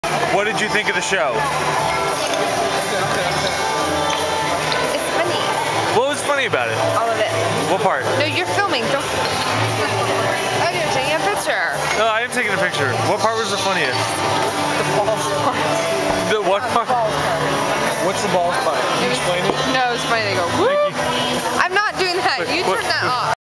2009.05 - Cancun Trip 2009 - Jenn After Some Drinks
Jenn had some drinks before, during, and after the show.  Here's her take on the show.